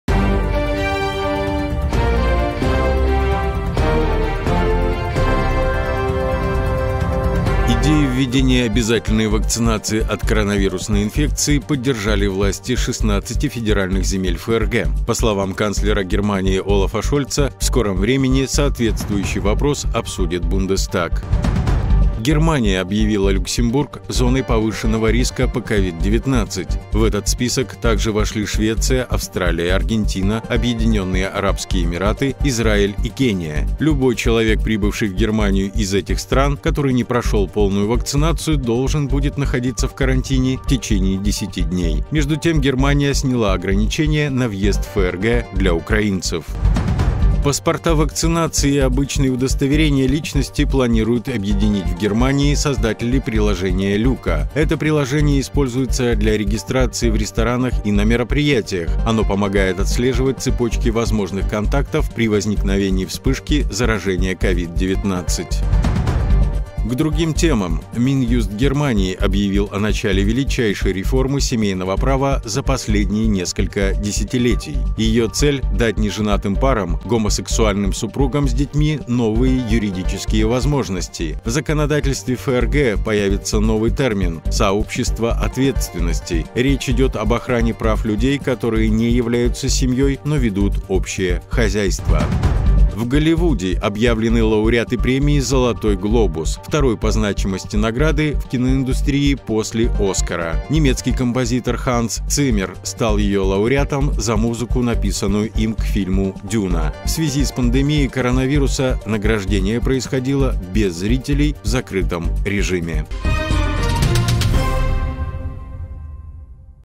Минюст Германии объявил о начале «величайшей реформы семейного права».
Новости со всей Германии каждый день на tvrus.eu